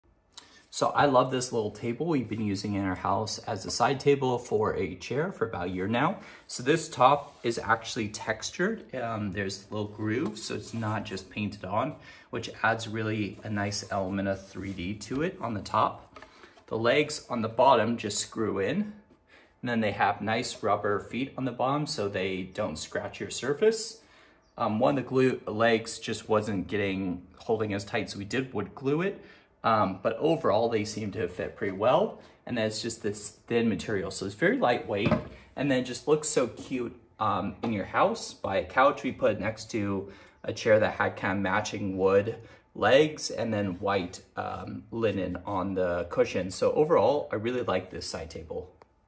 A close up view of the hanobe small round side table
Take a closer look at this modern farmhouse style small round side table found on amazon